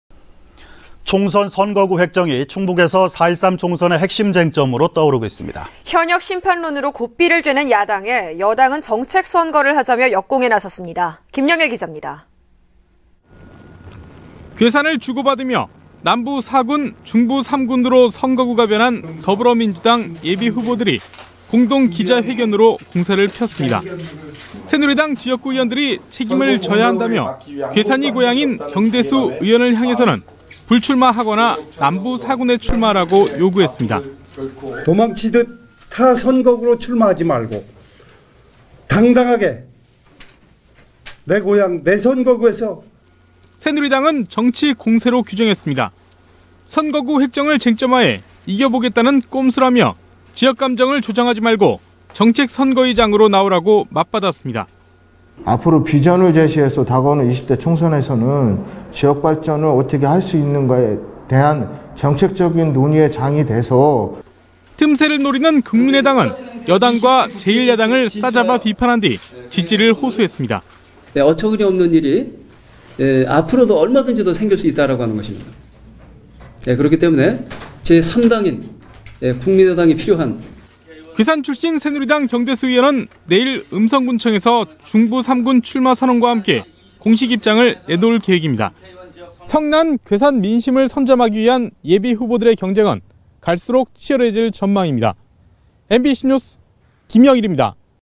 [청주MBC뉴스] '심판론' vs '책임전가'
◀앵커▶
 총선 초반 충북 쟁점으로 선거구 획정이
도마에 올랐습니다. 현역 심판론 고삐를 죄는 야당에 여당은 정책 선거를 하자며 역공에
나섰습니다. 김영일 기자입니다.
           ◀ＥＮＤ▶

           ◀ＶＣＲ▶
 괴산을 주고 받으며 남부 4군, 중부 3군으로
선거구가 변한 더불어민주당 예비후보들이
공동 기자회견으로 공세를 폈습니다.

 새누리당 지역구 의원들이 책임을 져야 한다며 괴산이 고향인 경대수 의원을 향해서는
불출마하거나 남부 4군에 출마하라고
요구했습니다.

           ◀ＩＮＴ▶
    임해종/더민주 중부3군 예비후보

 새누리당은 정치공세로 규정했습니다.

 선거구 획정을 쟁점화해
이겨보겠다는 꼼수라며 지역감정을
조장하지 말고 정책 선거의 장으로
나오라고 맞받았습니다.

           ◀ＩＮＴ▶
      이규석/새누리 사무처장

 틈새를 노리는 국민의당은
여당과 제1야당을 싸잡아 비판한 뒤
지지를 호소했습니다.

           ◀ＳＹＮ▶
      김영국/국민의당 예비후보
          "3당 필요하다"

 괴산 출신 새누리당 경대수 의원은
내일(오늘) 음성군청에서 중부 3군 출마선언과
함께 공식입장을 내놓을 계획입니다.

(S/U)
"성난 괴산의 민심을 선점하기 위한
예비후보들의 경쟁을 갈수록 치열해지고
있습니다. MBC뉴스 김영일입니다.